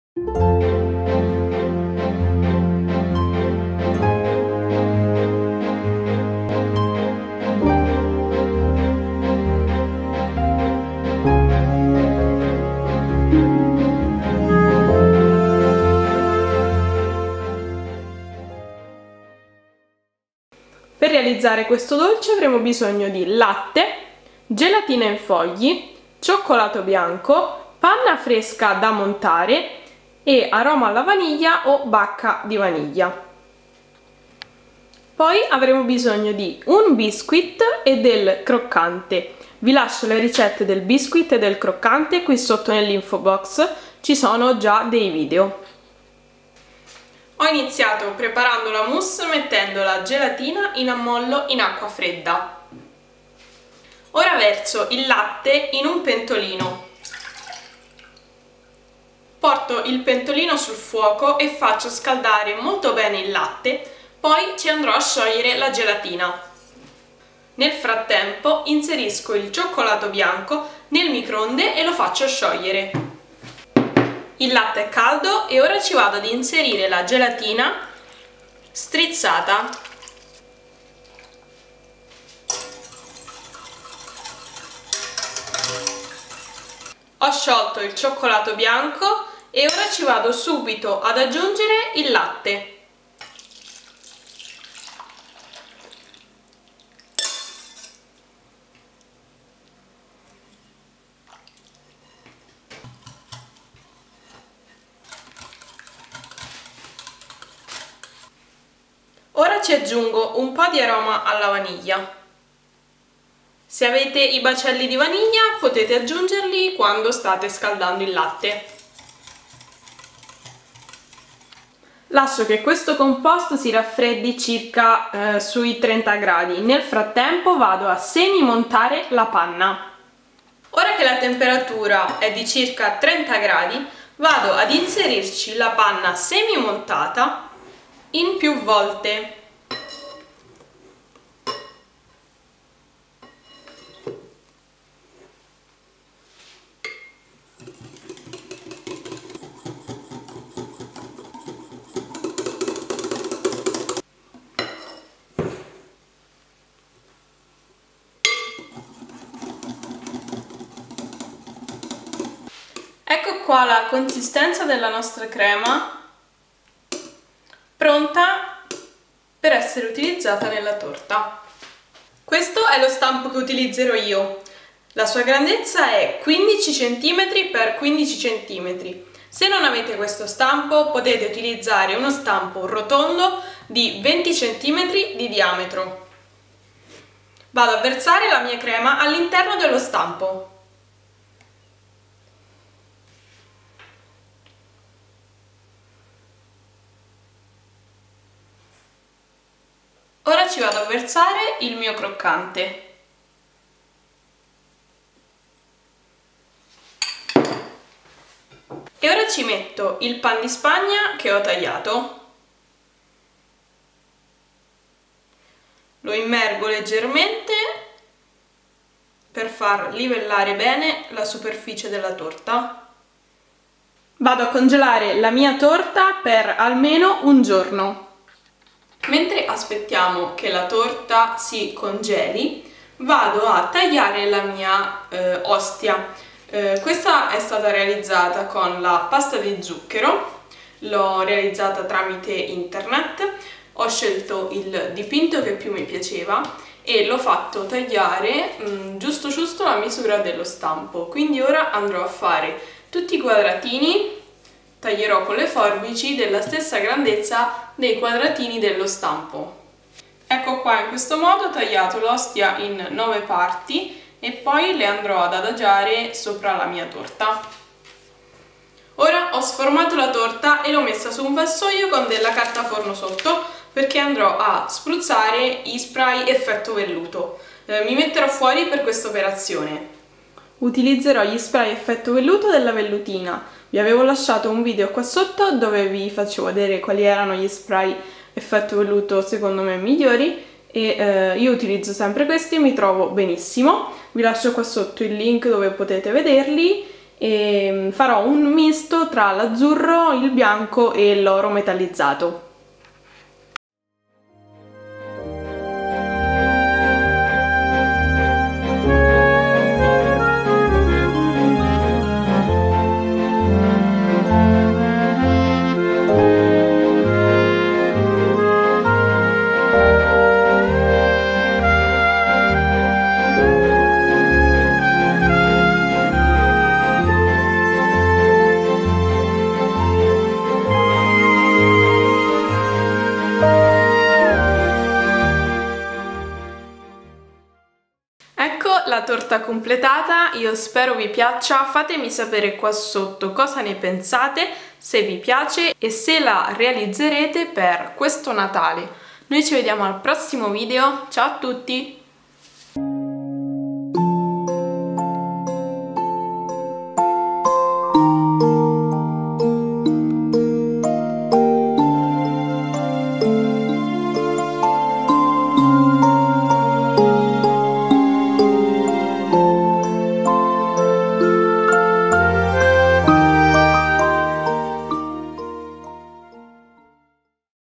Torta di Natale moderna con dipinto
INGREDIENTI MOUSSE ALLA VANIGLIA:
-260 gr di latte intero
-8 gr di gelatina in fogli
-360 gr di cioccolato bianco
-360 gr di panna semi montata

STAMPO UTILIZZATO: 15 CM X15 CM
In sostituzione si può usare uno stampo rotondo da 20 cm di diametro e mettere l'ostia di pasta di zucchero intera.